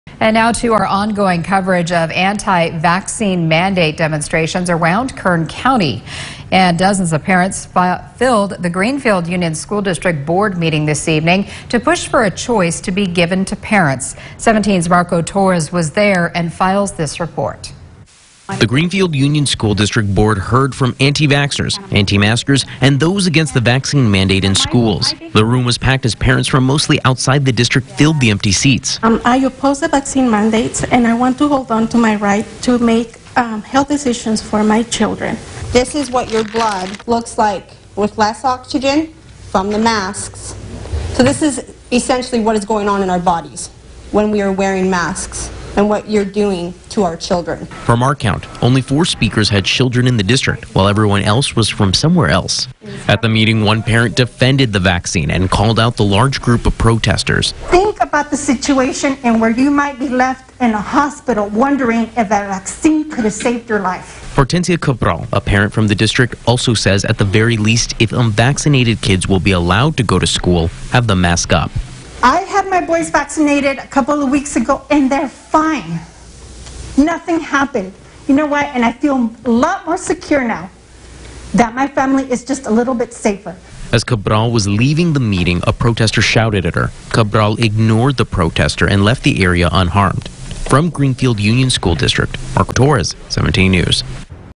Parents speak out in support, against the vaccine mandate at Greenfield Union School District board
Parents speak out in support, against the vaccine mandate at Greenfield Union School District board meeting